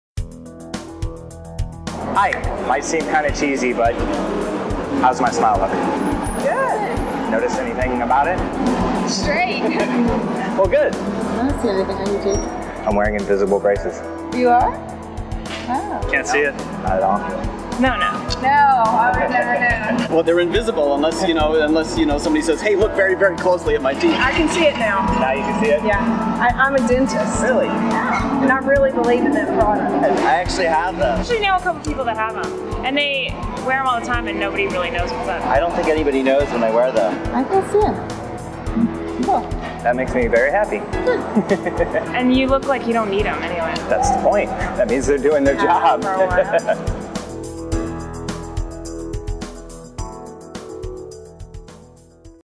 Can you tell If I'm wearing Invisalign® braces?
Invisalign Braces are a clear alternative to traditional braces.